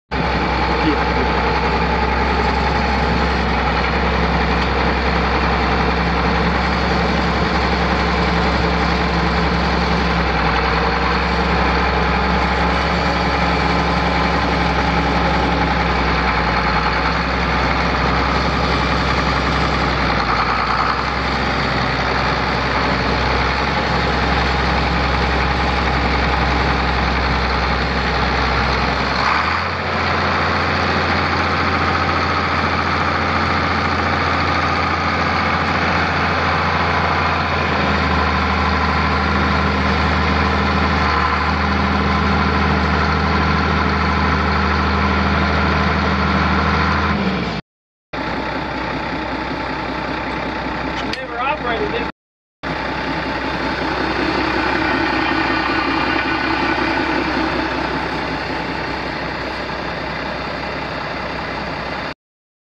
1975 Case 1175 tractor for sale at auction | bidding closes February 2, 2022
1975 Case 1175  tractor
 Hours: 7,569 on meter
 Serial: 8779147
Engine
 Cylinders: 6
 Fuel type: Diesel
Transmission
 Manual
 8F - 2R
Operators station
 AC, Heat
Operational
 PTO: 540, 1000
 Three point
 Hydraulic remote sets
 Rear: 2
 Suitcase weights: 4
Tires
 Front: 11.00-16
 Rear: 18.4-38
 Wheel weights: 2
It's located in Coweta, OK, and we're selling it to the highest bidder regardless of price.